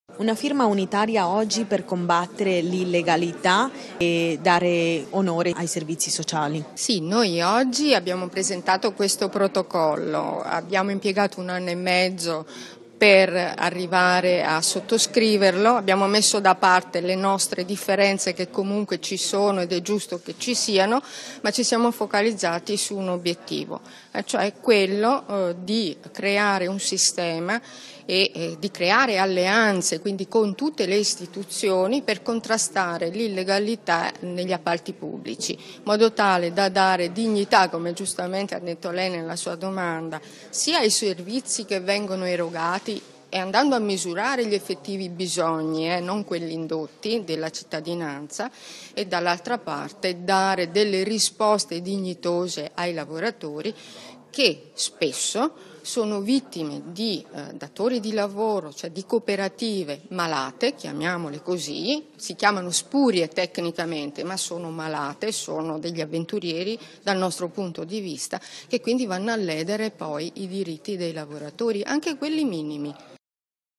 COOPERATIVE E SINDACATI:INSIEME PER LA LEGALITA' NEL SOCIALE
Contrastare l'illegalità e il criterio del massimo ribasso nell'affidamento dei servizi da parte degli enti pubblici, t... [continua a leggere su